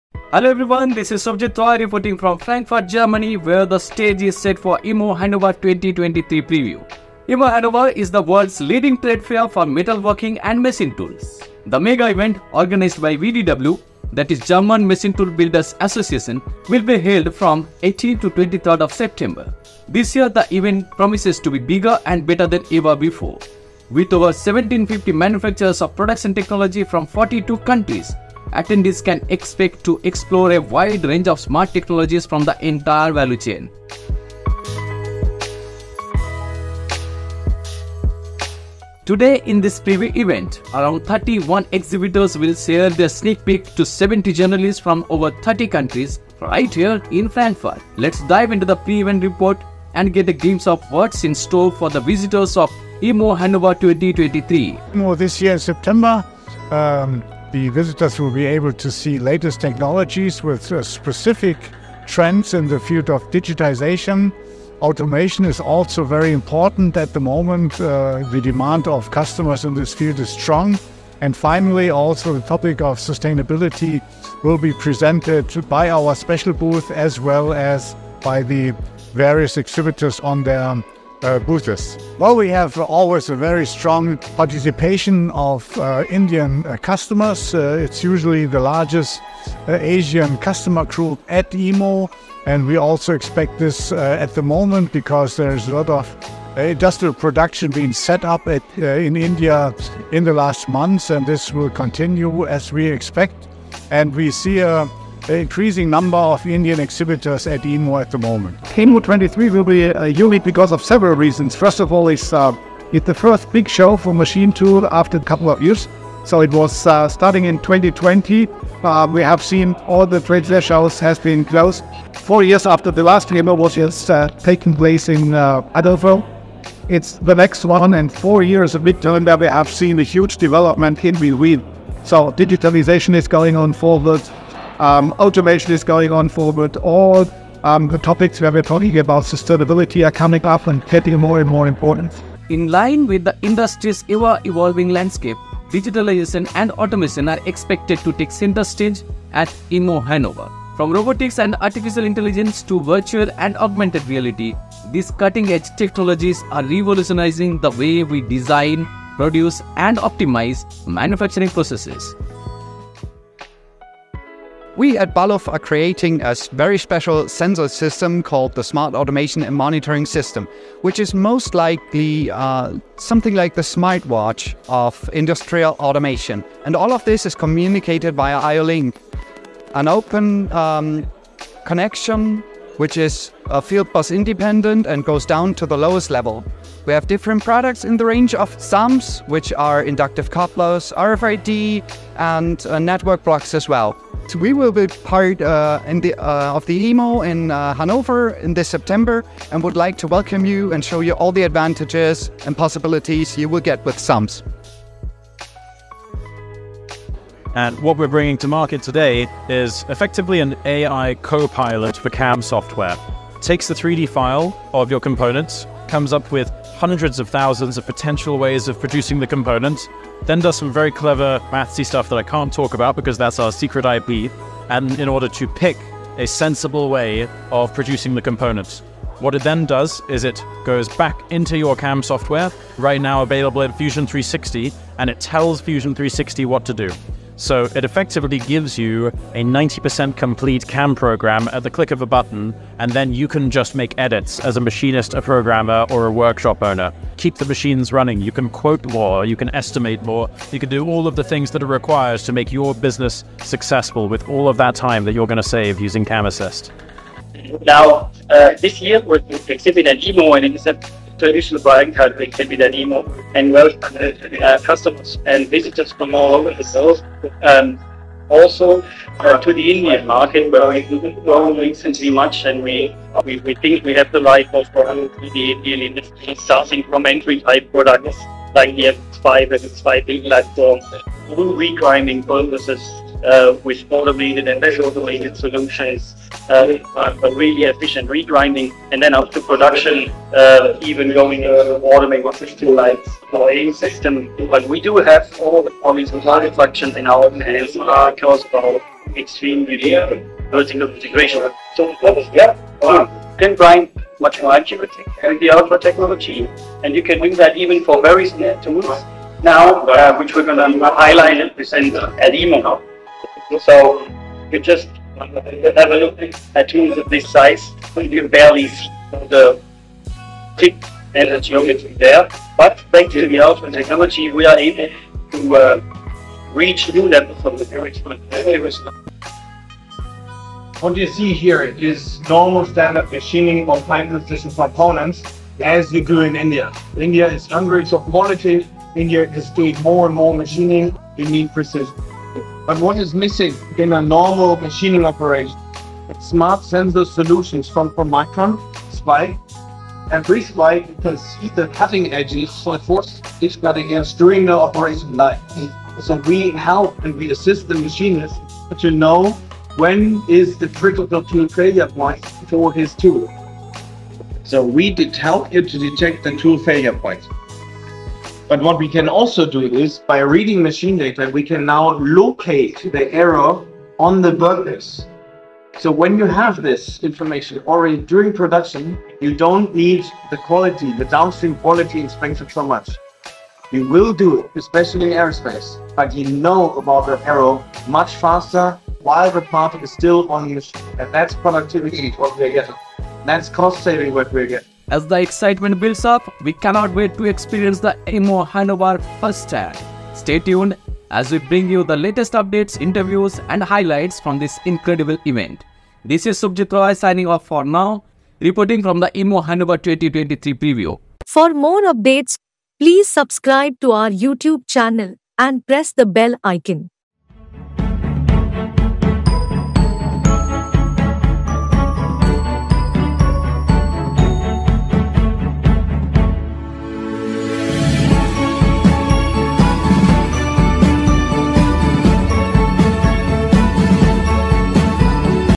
EMO Hannover 2023 | Exclusive Preview
EMO Hannover is the world's leading trade fair for metalworking and machine tools. The mega event, organized by VDW i.e. German Machine Tool Builders' Association, will be held from 18 to 23 September.

This year, the event promises to be bigger and better than ever before. With over 1,750 manufacturers of production technology from 42 countries, attendees can expect to explore a wide range of smart technologies from the entire value chain.

In a preview event held in Frankfurt, around 31 exhibitors shared their sneak peek to 70 journalists from over 30 countries.

Let's dive into the pre-event report and get a glimpse of what's in store for the visitors of EMO Hannover 2023.

Featuring:
0:00 - INTRO
0:56 - Dr. Wilfried Schäfer, Executive Director, VDW

1:50 - Dr. Markus Heering, Executive Director, VDW

2:44 - Torsten Delfs, Product Manager, Balluff

3:26 - Theo Saville, CEO, CloudNC
4:18 - Joachim Jäckl, European Marketing & Communications Manager, ANCA
5:30 - Lennart Riehle, Head of Sales & Marketing (Tool Control – spike), pro-micron GmbH

Image Courtesy: VDW,  Deutsche Messe AG